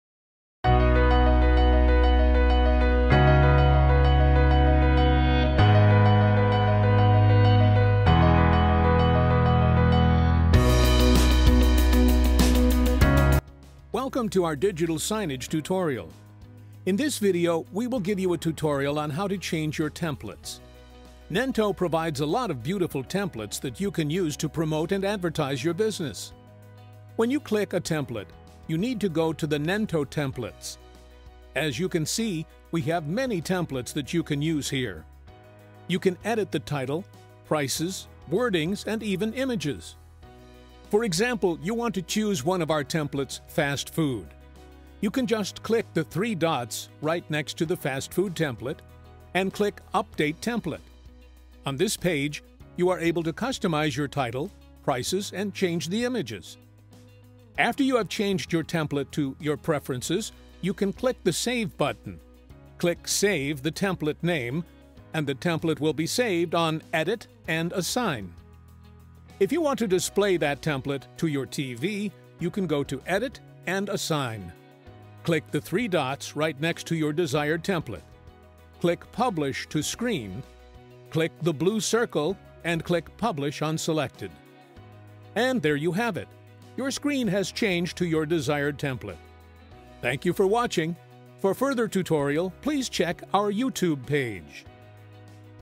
Change Your TV Screen with Interactive Templates | Nento Digital Signage
Welcome to Nento Digital Signage Tutorial.
In this video, you will be able to change and customize templates.
Nento provides interactive, eye catching and customizable templates that you can use to display your products or menus on your tv screens. We will guide you step by steps on how you can modify each template with your own content and publish it to your tv screen.